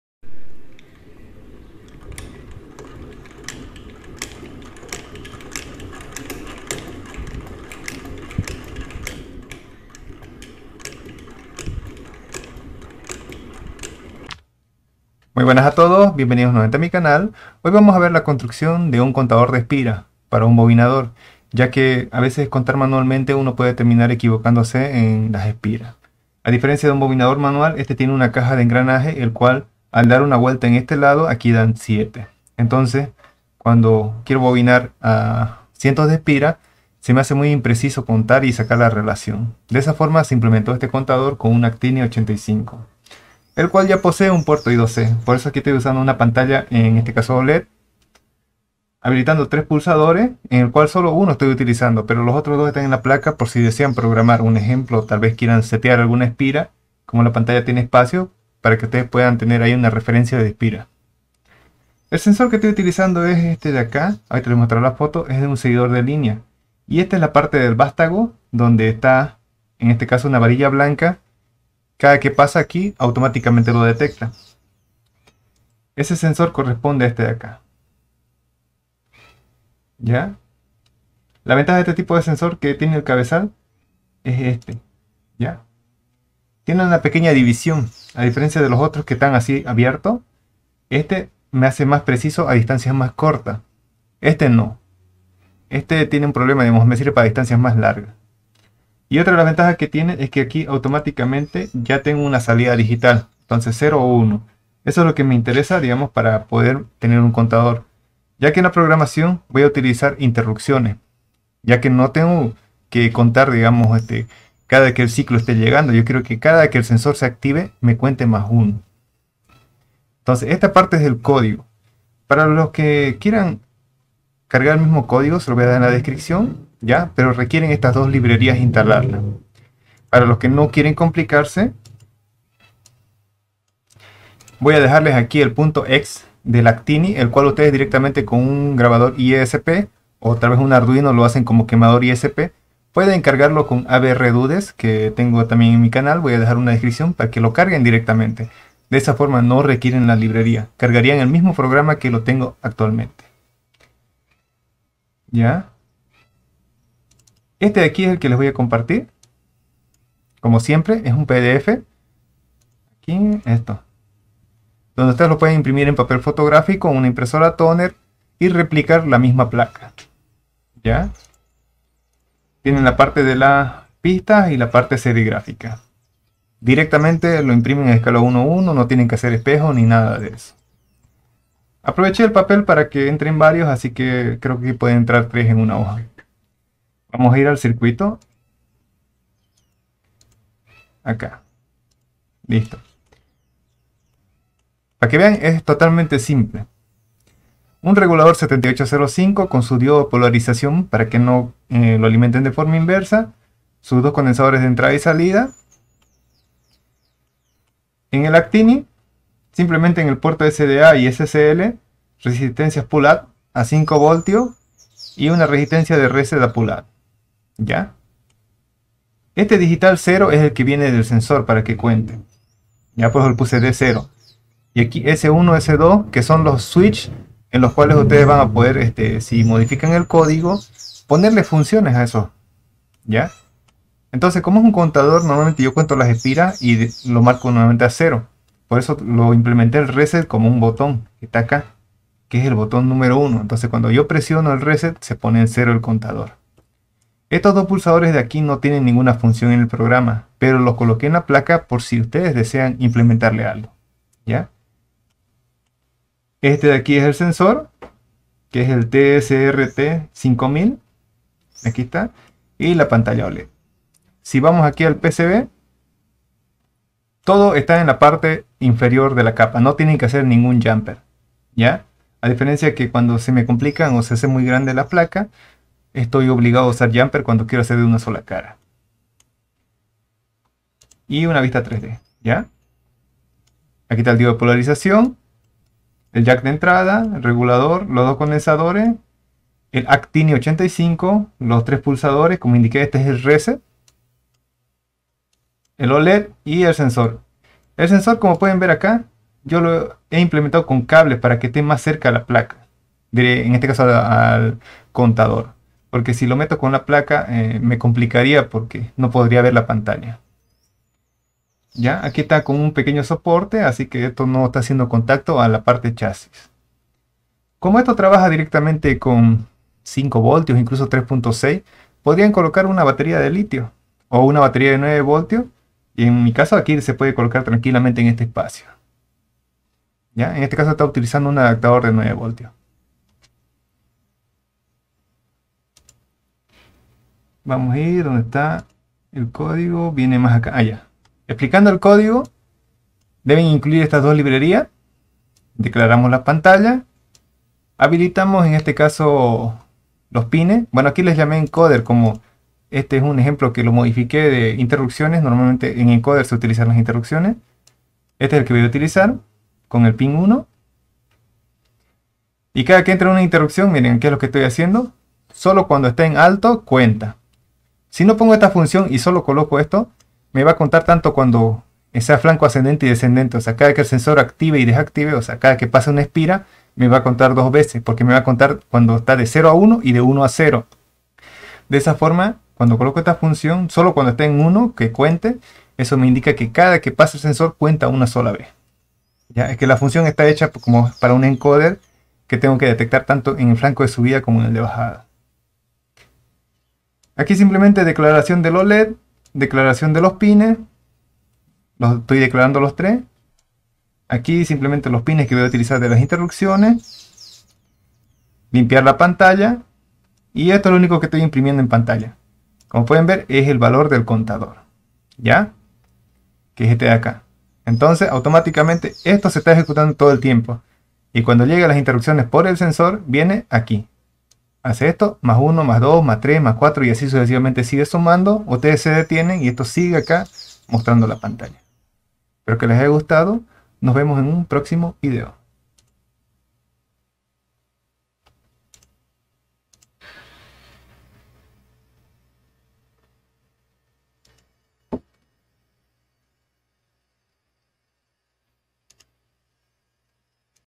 ⚡ Construye tu mismo tu Contador de espiras para bobinador con ATtiny85 Oled 0.91 y modulo TCRT5000
Siendo MIEMBRO podrás ver los videos antes de ser PUBLICADOS ;)